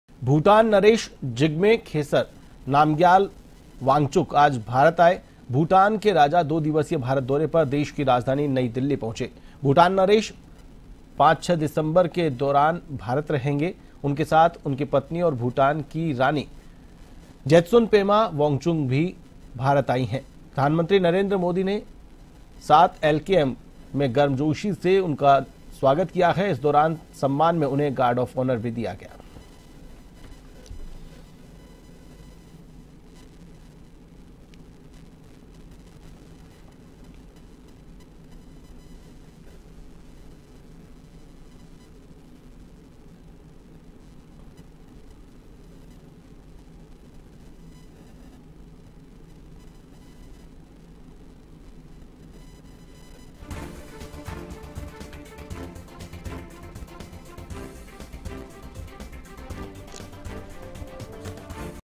Delhi: भूटान नरेश अपनी पत्नी के साथ भारतीय प्रधानमंत्री PM Narendra Modi से मिलने पहुंचे | A1TV
a1tv delhi bhutan pmnarendramodi delhines delhiairport Delhi: भूटान नरेश अपनी पत्नी के साथ भारतीय प्रधानमंत्री PM Narendra Modi से मिलने पहुंचे | A1TV